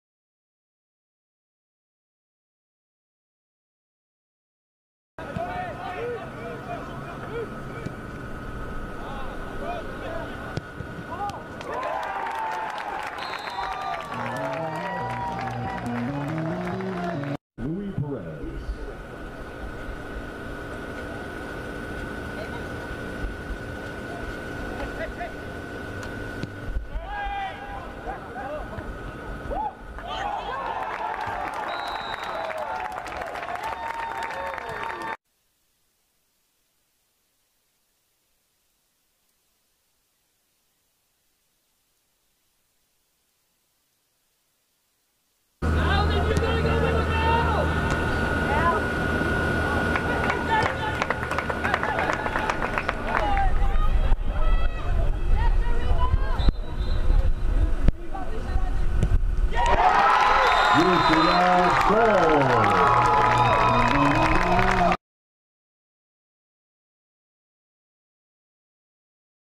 Plays of the Game, UCF vs. UTRGV
The Knights defeated the UTRGV Vaqueros by a score of 3-2 in double overtime on September 8, 2019. Relive the top plays, including Yoni Sorokin's game winning penalty kick goal.